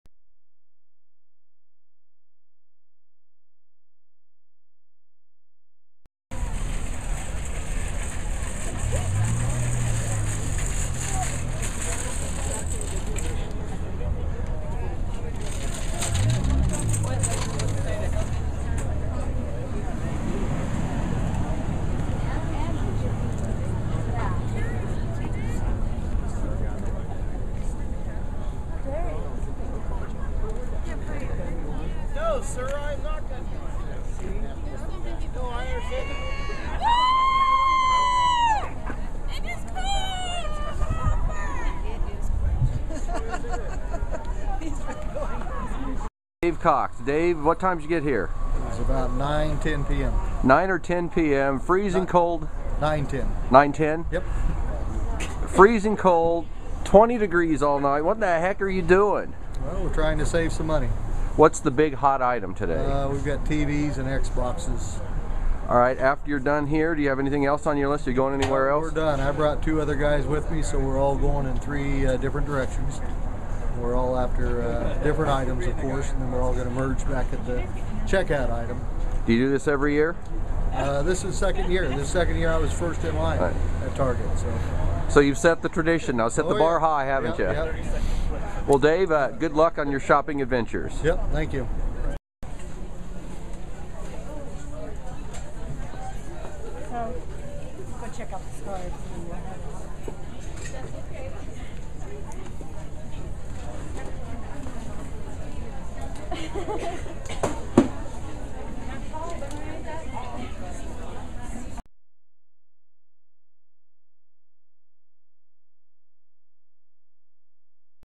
Black Friday 2010
Shoppers line up outside the Target store in Salina.  Dave Cox from Wilson was fist in line, lining up just after 9:00 the night before.  Shoppers shop inside the store.